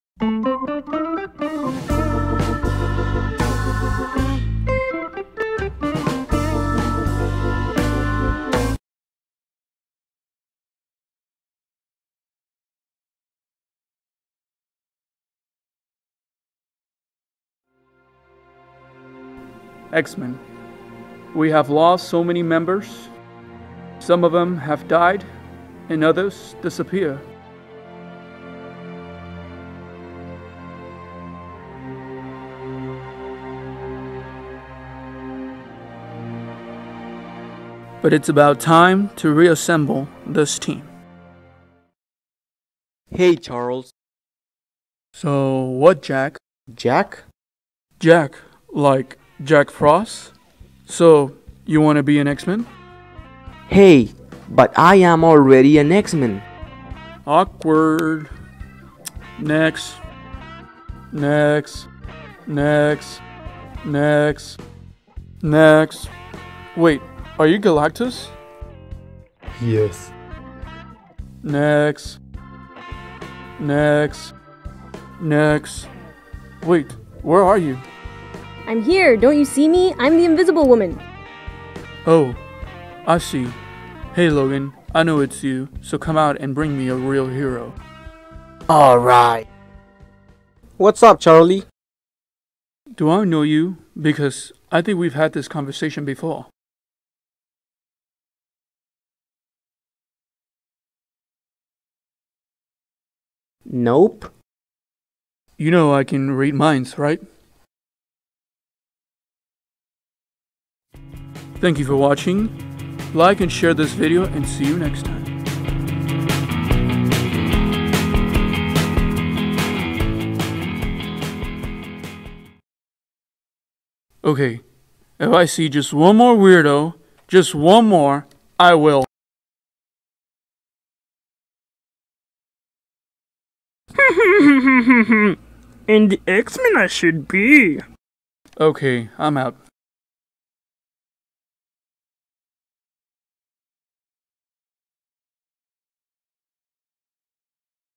What Happened to Charles After The Last Stand
Have you ever asked yourself what really happened to Charles Xavier after the 3rd movie? Well. we have the answer in a funny and really entertaining perspective.